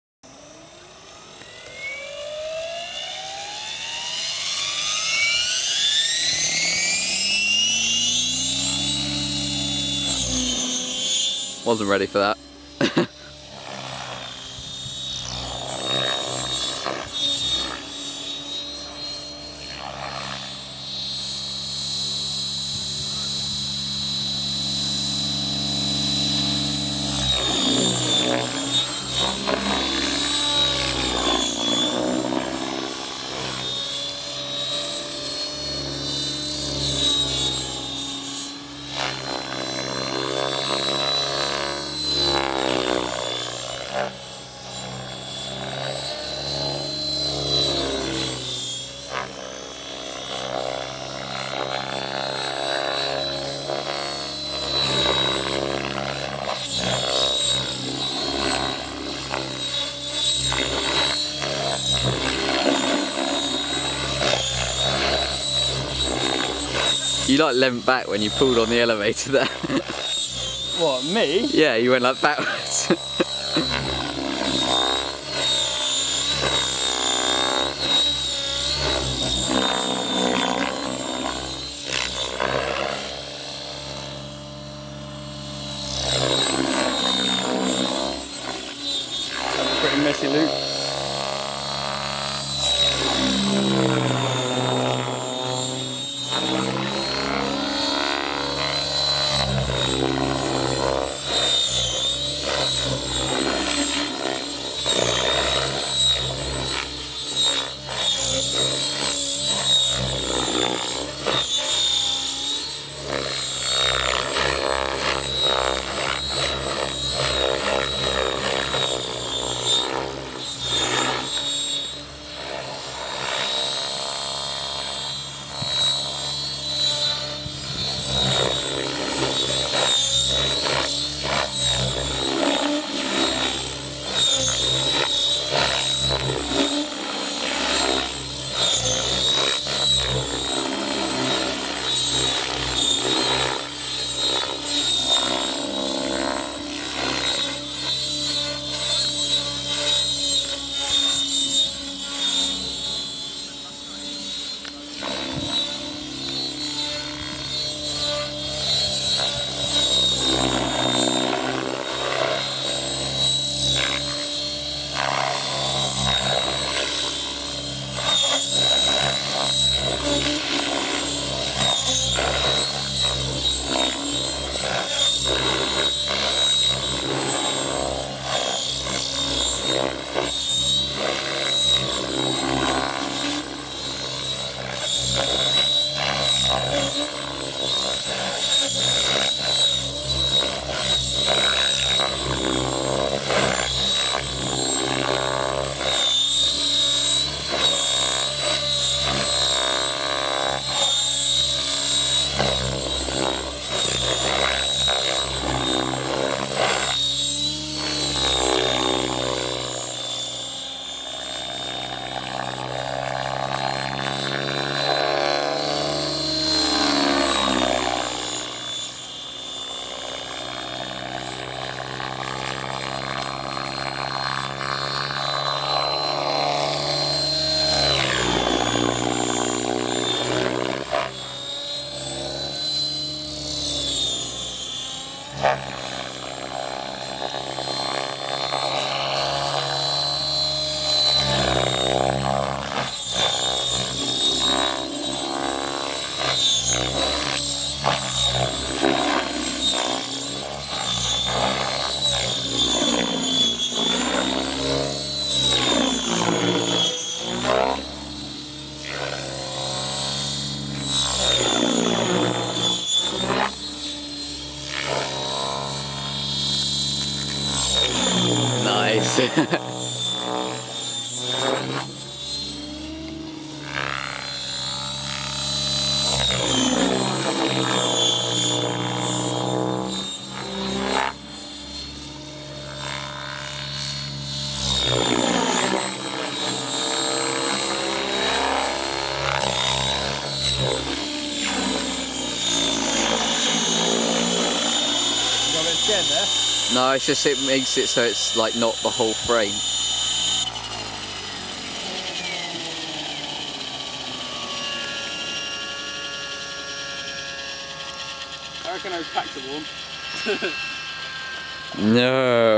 RCHA Fun Fly - TREX 700E 3D Pilot: Tom Stanton
Flying the 700E at the RCHA Fun Fly